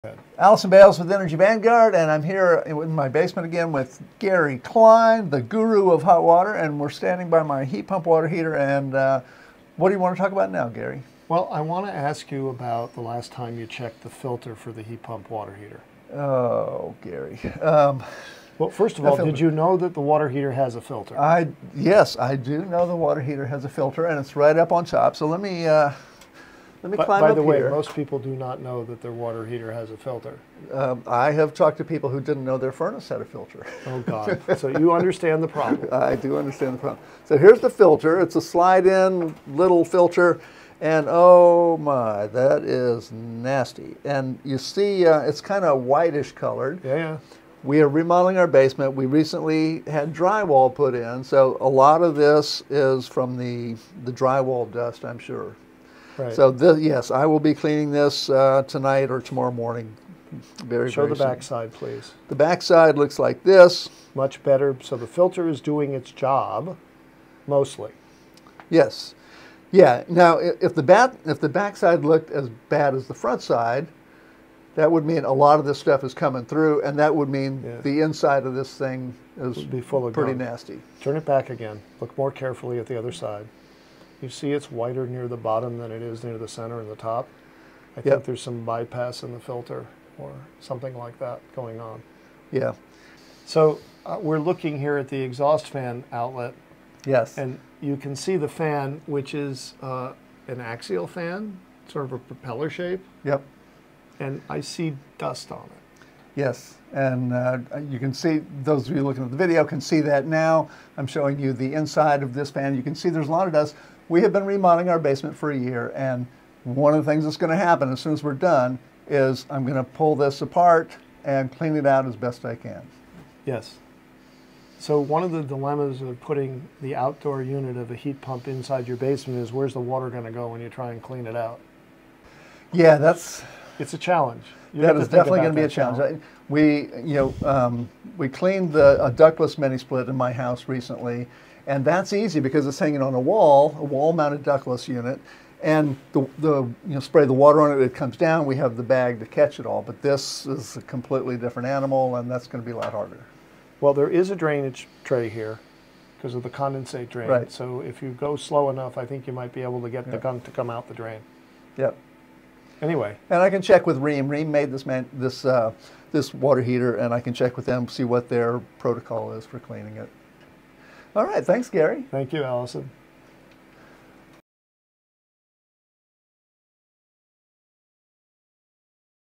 Filtering the Air in a Heat Pump Water Heater, Part 1
A heat pump water heater pulls in air to get heat for the water.  That air needs to be filtered.  In this short video, Gary Klein and I take a look at the filter in my heat pump water and discuss the issues.

In Part 2, we'll talk about a better way to filter the air in this type of water heater.